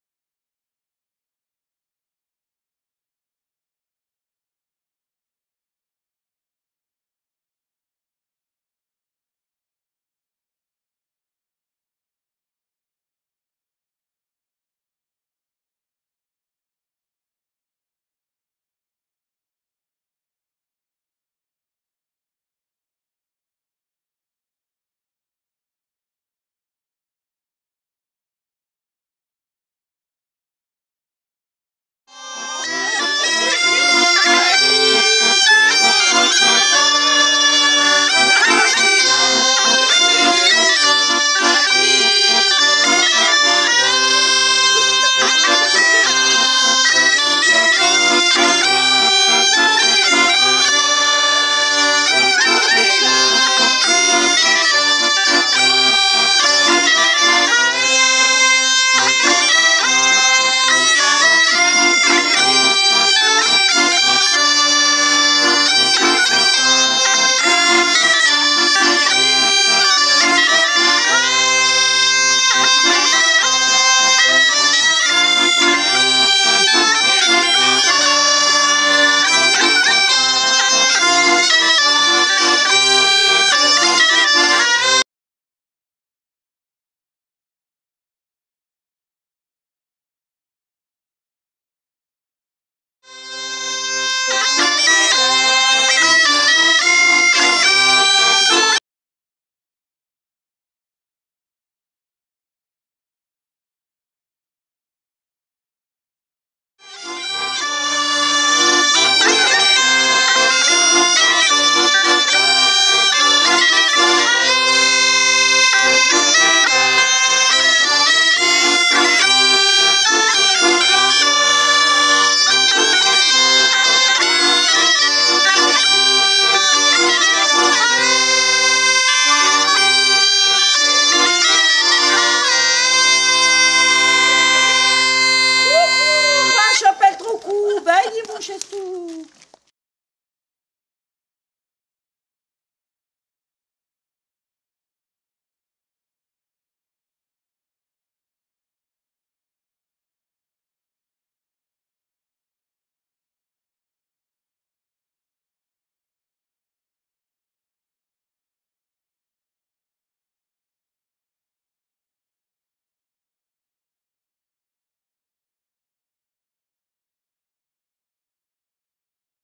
Les Réveilhés à Ytrac.mpg
Les réveilhés, chanter tout les soirs ,une semaine avant les rameaux, par le groupe la Yoyette d'ytrac .